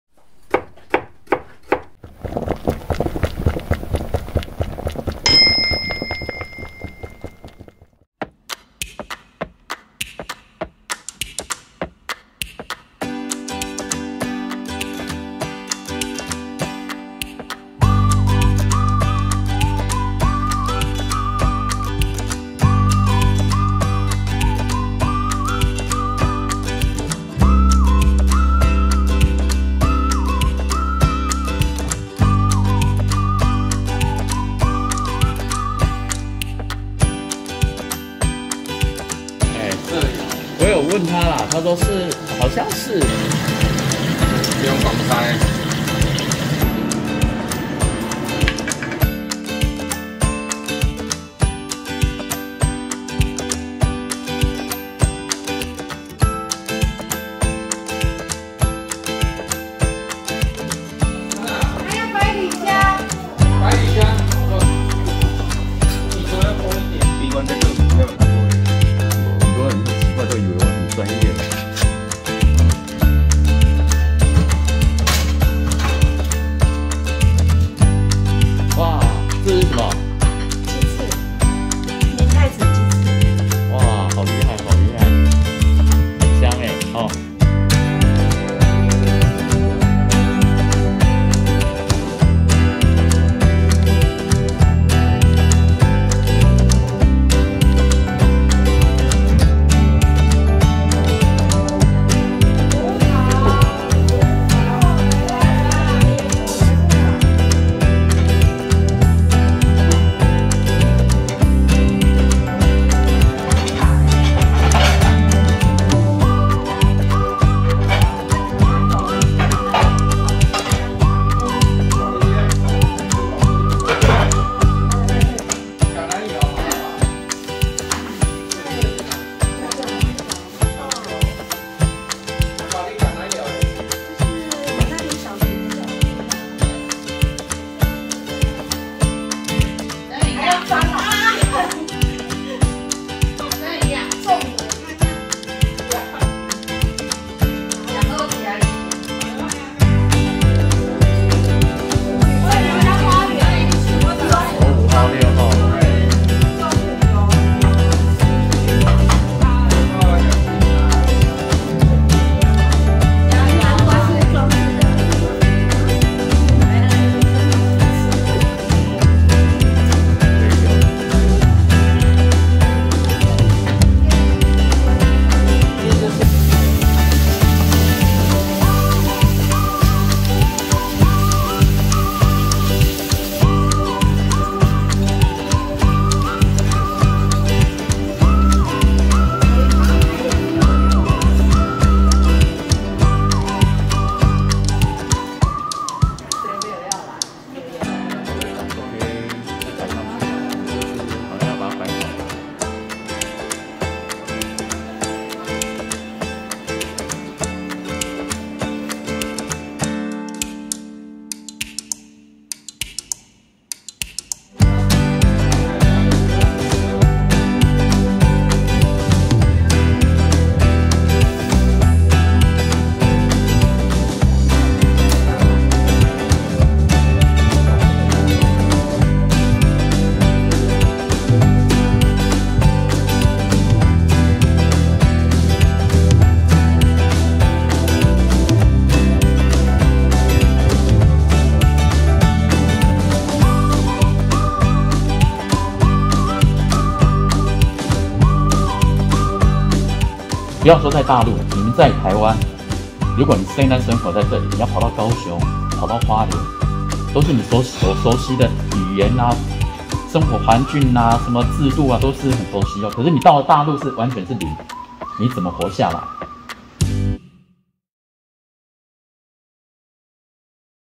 Lillanの私人厨房___生活藝術家阿寶
Lillanの私人厨房___生活藝術家阿寶，從我認識她開始，她從少女升格為媽媽，再從媽媽成為生活家。她隨時充實自己，把生活變得很藝術，更難得她有一位好老公，她想要個壁爐過幾天就出現了！她想在梅花樹下品茗，沒多久家裡花園就有棵梅花樹，看似她總有那麼多浪漫的要求，老公總不厭其煩做給她，好像她隨時都有不同的要求，其實是她想帶給一家人浪漫的生活。所以總會為家人和好朋友做豐盛的西式料理，讓大家除了吃美食還品味生活。